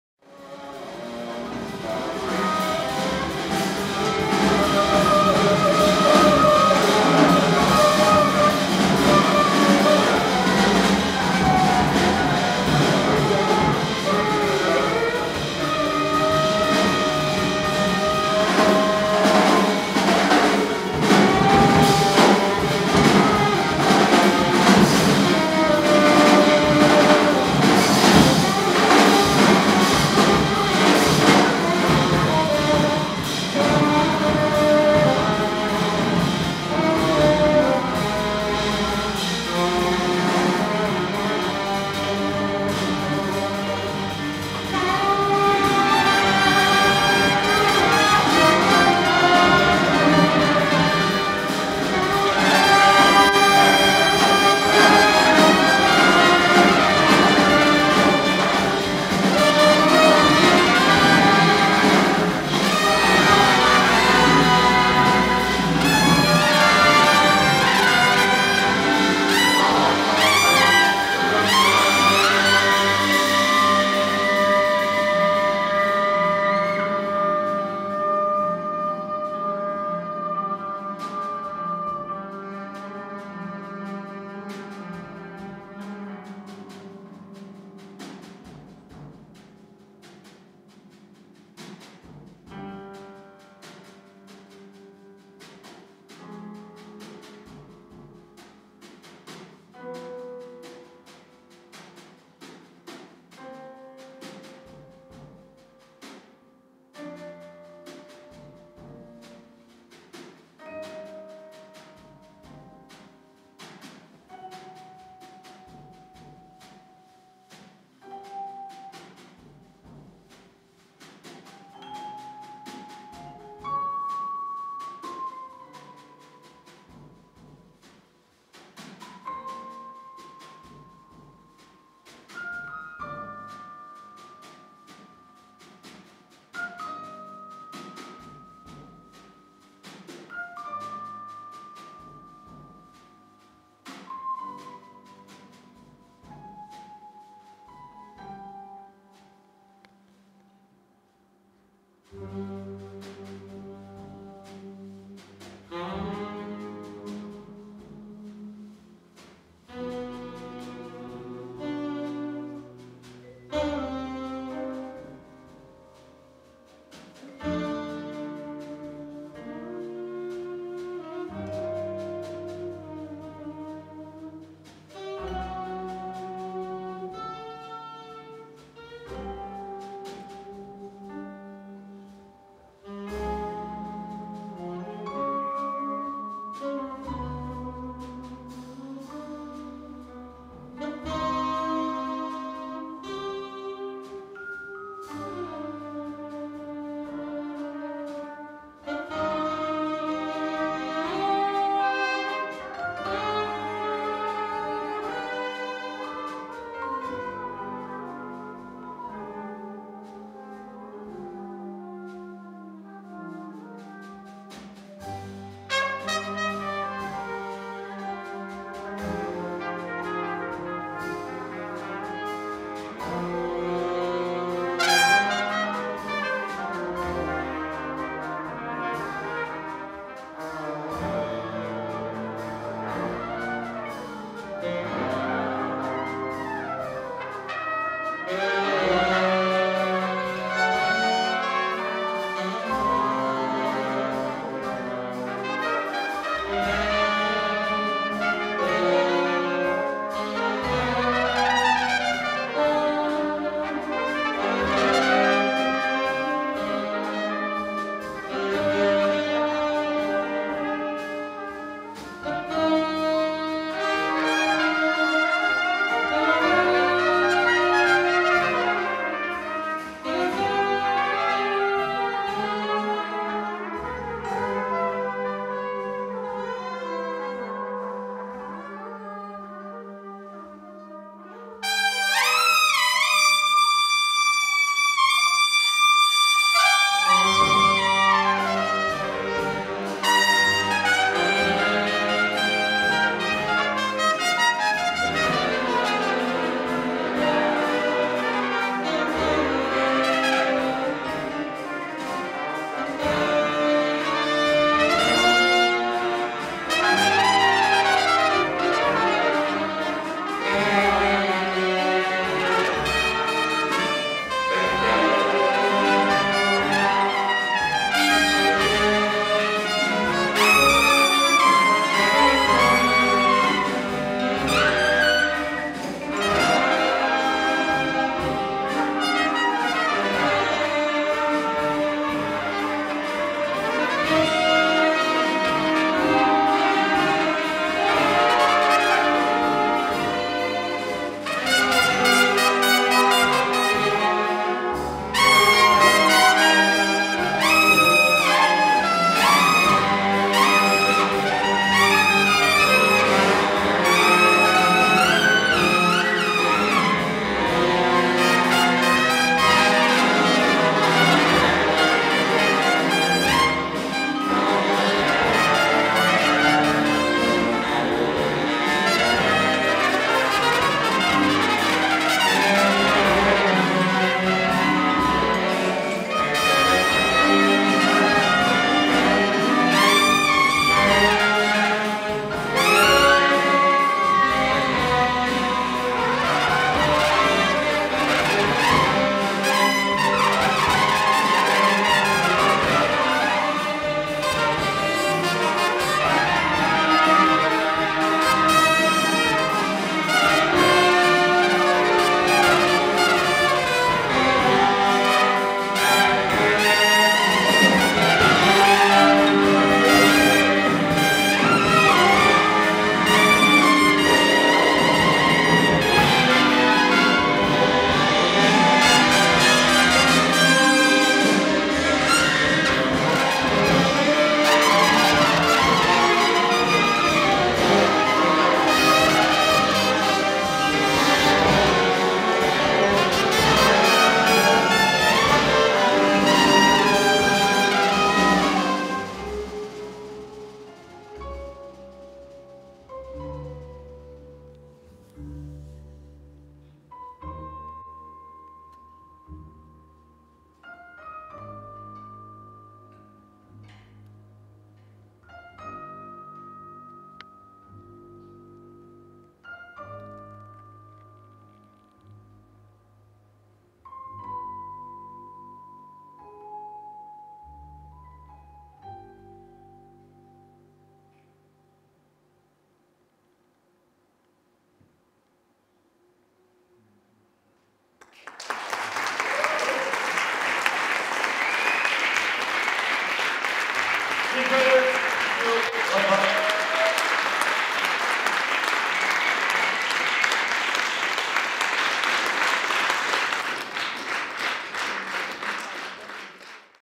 V Lublin Jazz Festiwal Martin Küchen's Angles 9
Martin Küchen's Angles 9

Goran Kajfes- trumpet
Magnus Broo -- trumpet
Eirik Hegdal -- baritone + soprano saxophone
Mats Äleklint- trombone
Martin Küchen -- alto saxophone, leader
Johan Berthling -- bass
Alexander Zethson -- piano
Mattias Ståhl -- vibraphone
Kjell Nordeson/Andreas Werliin- drums

Martin Küchen -- szwedzki saksofonista mający opinię jednego z najbardziej niezwykłych improwizatorów w Europie, wirtuoza saksofonu, odważnego i bezkompromisowego artysty, który równie dobrze radzi sobie w składach free-jazzowych, jak i w projektach skręcających w stronę ponad gatunkowej awangardy.